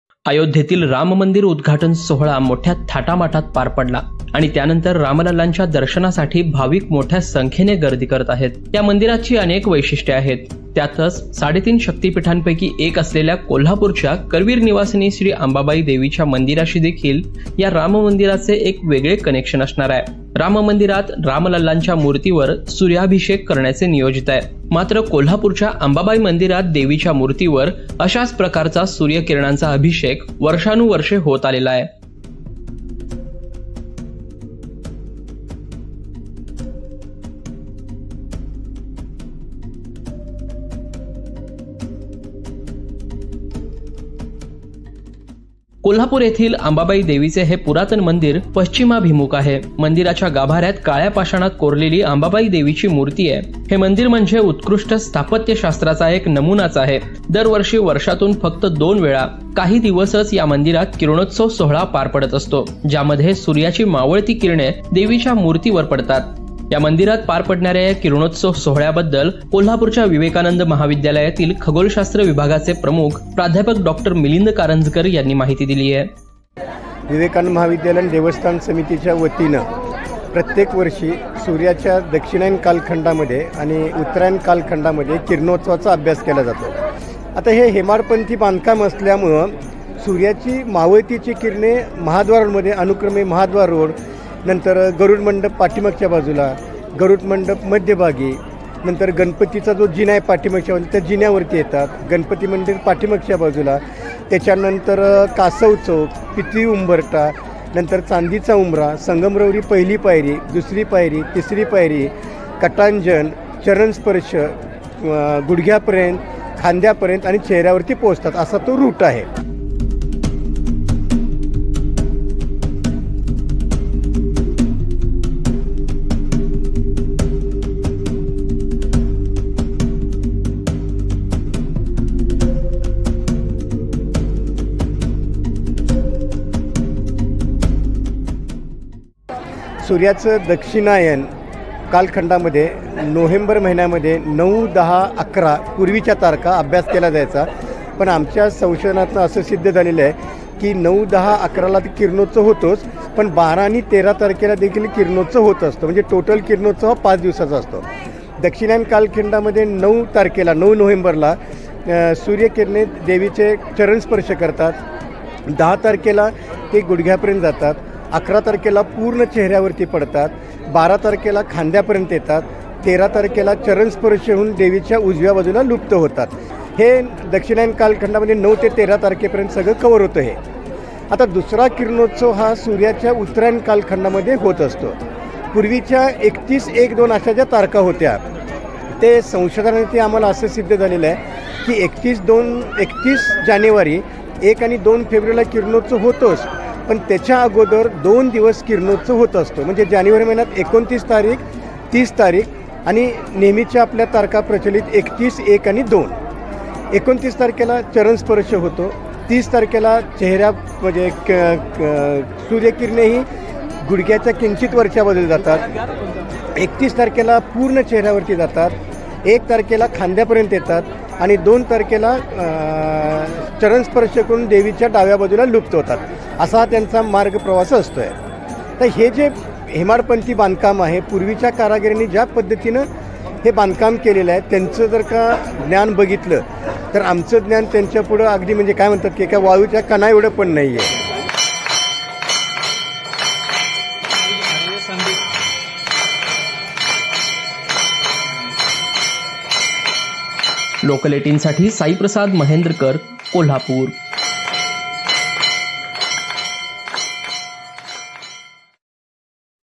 Ambabai Devi Kiranotsav अंबाबाई मंदिरात देवीच्या मूर्तीवर कसा होतो सूर्यकिरणांचा अभिषेक?
कोल्हापूर येथील अंबाबाई देवीचे हे पुरातन मंदिर पश्चिमाभिमुख आहे. मंदिराच्या गाभाऱ्यात काळ्या पाषाणात कोरलेली अंबाबाई देवीची 3 फूट उंच सुंदर अशी मूर्ती आहे. संपूर्ण मंदिराच्या बांधकामावेळी दिशांचा योग्य अभ्यास करून हवा-प्रकाशाचा अप्रतिम मेळ साधण्यात आला आहे. त्यामुळे हे मंदिर म्हणजे उत्कृष्ट स्थापत्यशास्त्राचा एक नमुनाच आहे.

This ancient temple of Ambabai Devi in ​​Kolhapur is facing west. In the core of the temple there is a beautiful 3 feet high idol of Goddess Ambabai carved in black stone. During the construction of the entire temple, a wonderful combination of air and light has been achieved by proper study of the directions. Therefore, this temple is an example of excellent architecture.